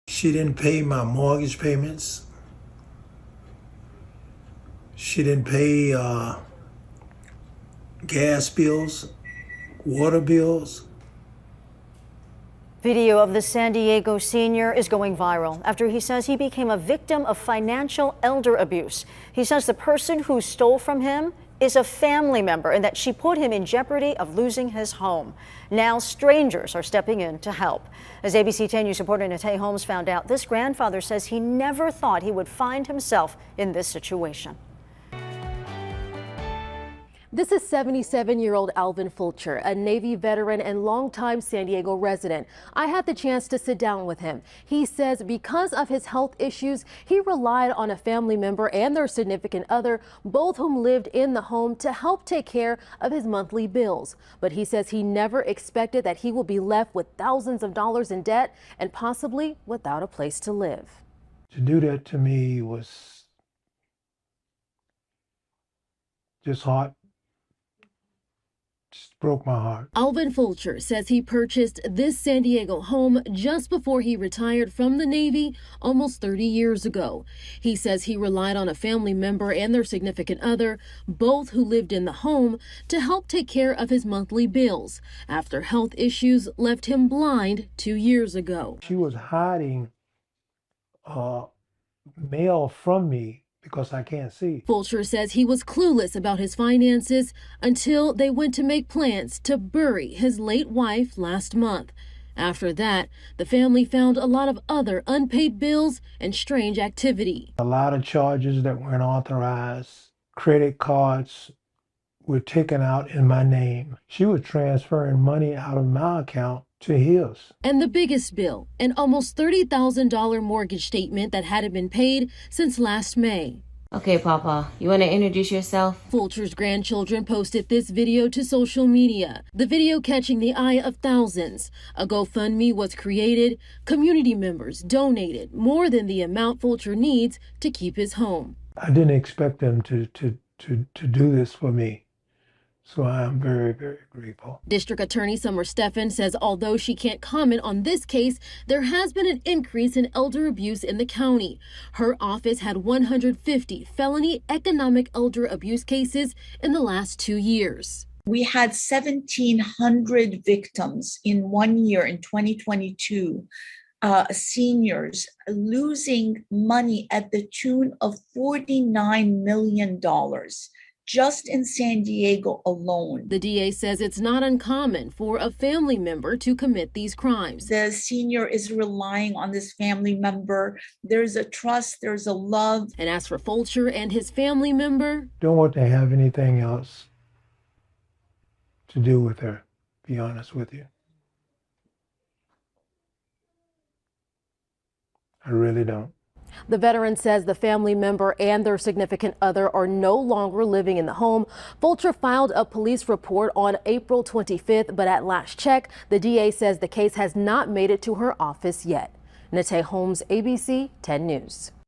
Viral TikTok and generous donations save financial elder abuse victim's home
A video of a San Diego man is going viral after he says he became a victim of economic elder abuse. He claims a family member stole his money putting him in jeopardy of losing his home.